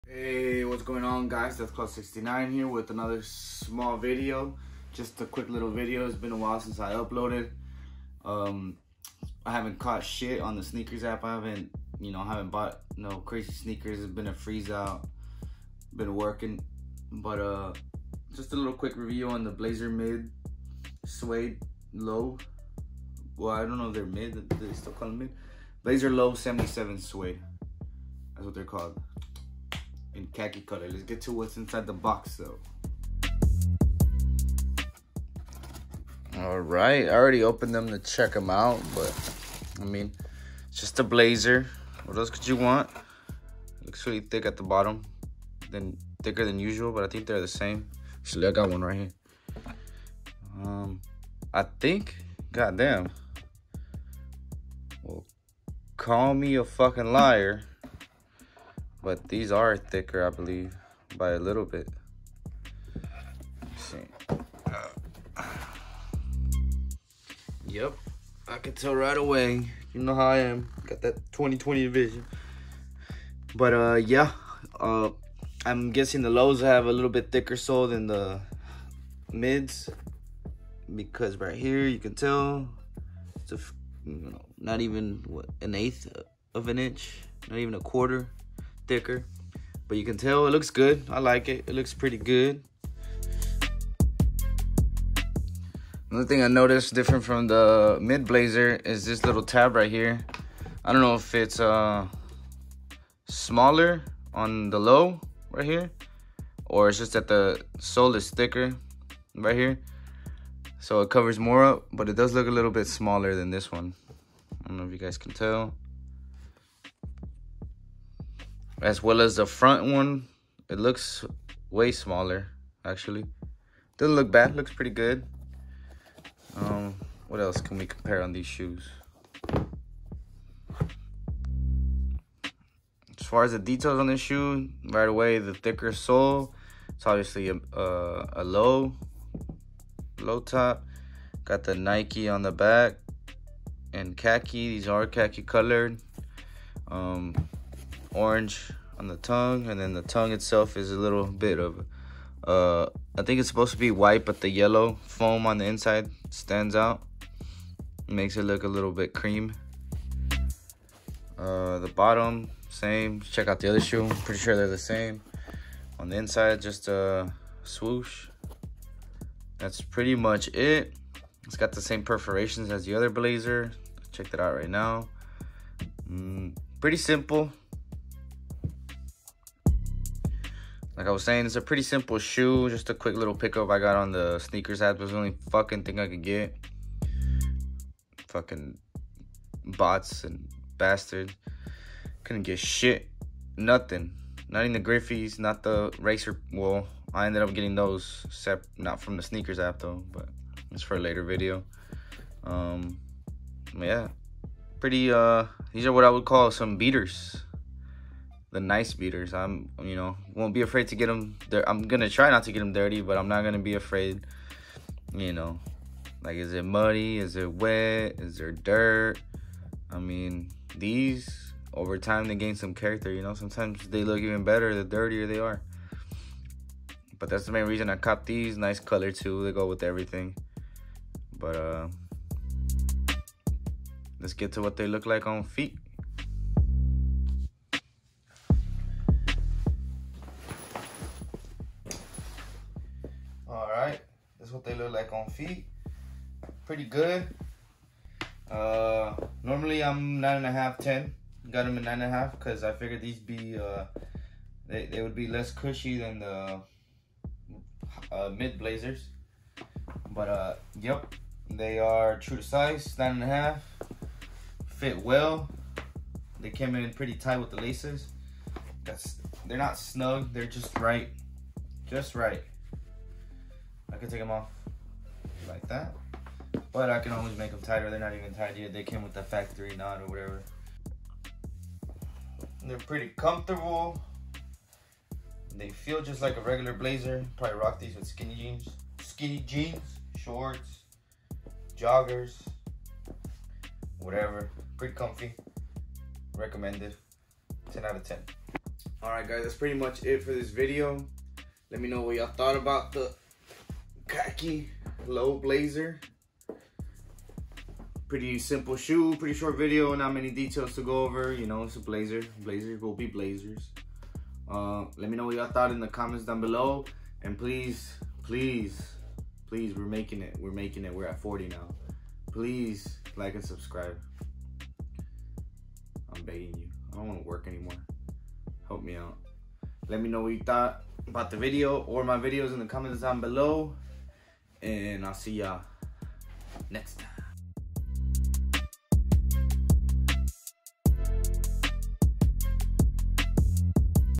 Blazer Low ‘77 Suede Medium Khaki unboxing and review
mediocre review on my latest tiny but humble snkrs W 😔 havent been able to cop anything on that app lately